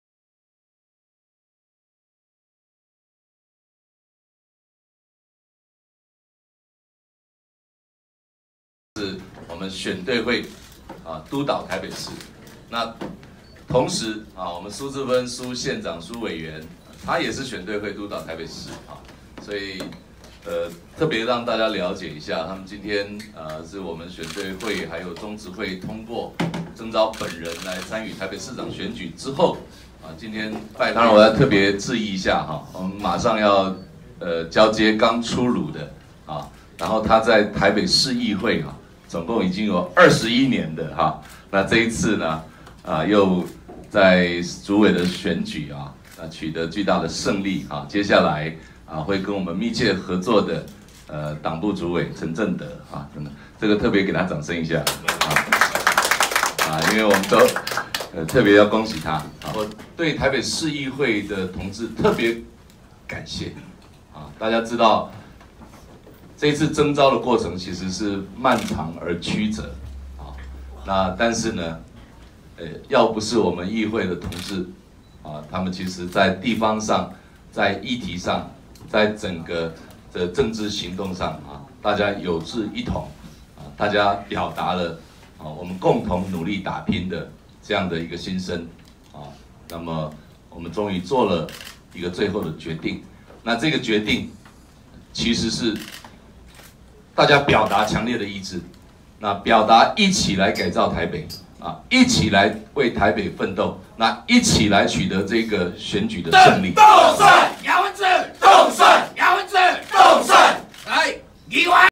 姚文智拜會北市議會民進黨團（記者鄭傑文）
民進黨台北市長參選人姚文智31日拜會台北市議會民進黨團，與市議員們一起呼喊「凍蒜」口號。